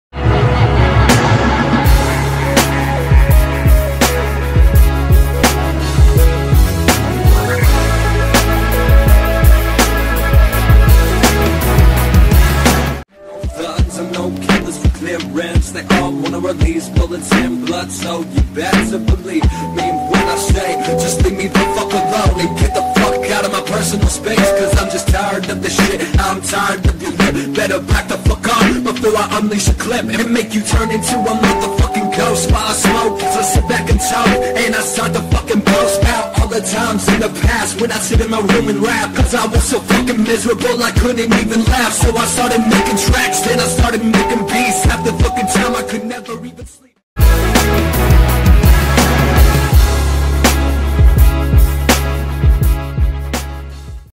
((Sumo) -Legacy- (@Life_Exhaled) Performs at Coast 2 Coast LIVE | Dallas All Ages Edition 11/11/17
GT of EarDrummers - Producer for Lil Wayne, Lil Uzi, RaeSremmurd, Young Thug, ASAP Ferg, Jim Jones, MGK & More

Saturday, November 11, 2017 9:00 PM